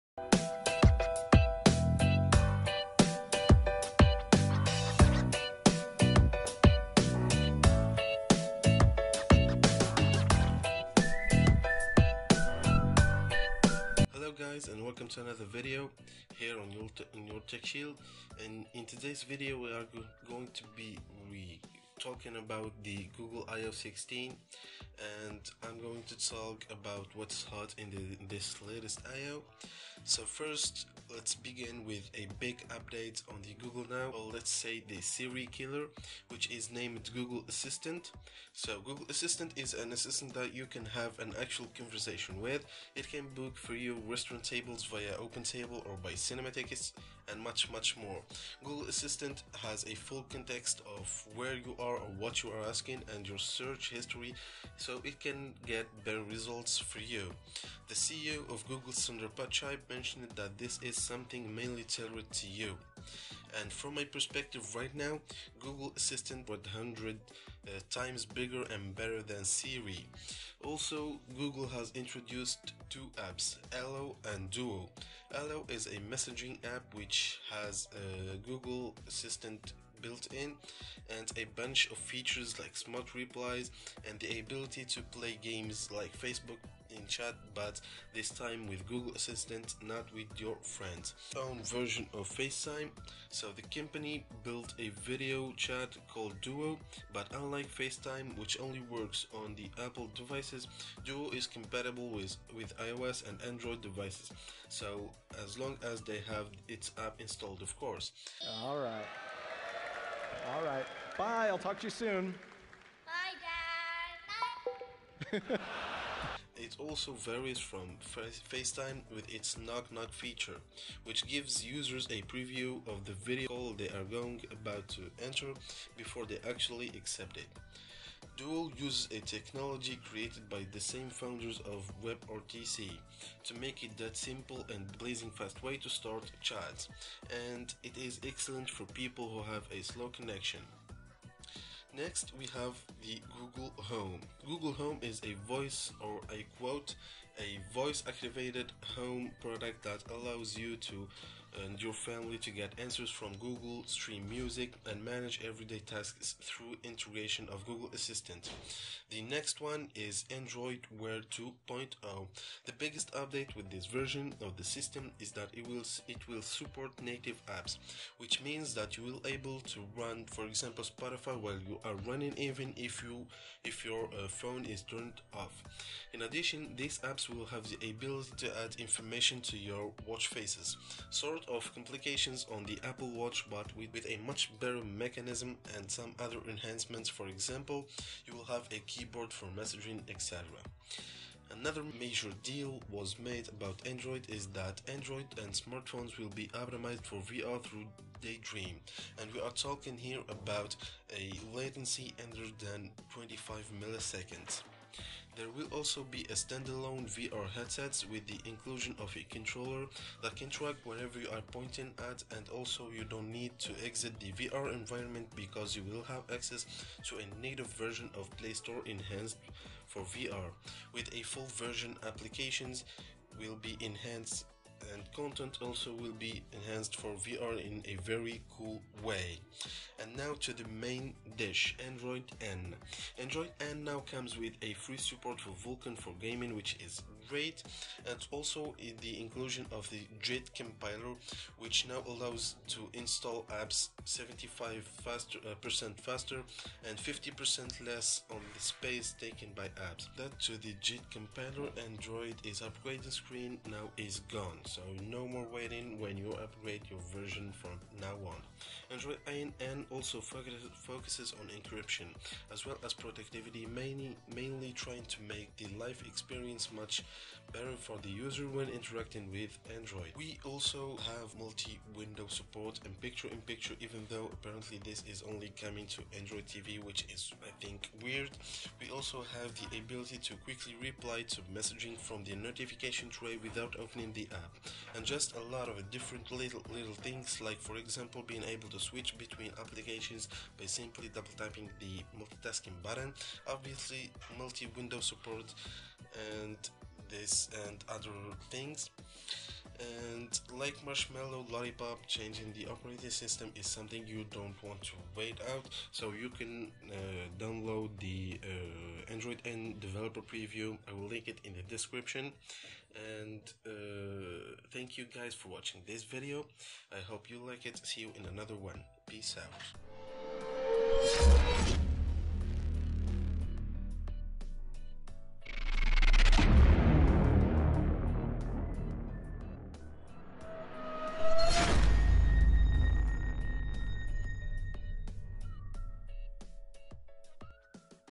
Google IO16 Recap (What's hot)
The recap of the latest Google Io 2016

Pre-register to Allo and Duo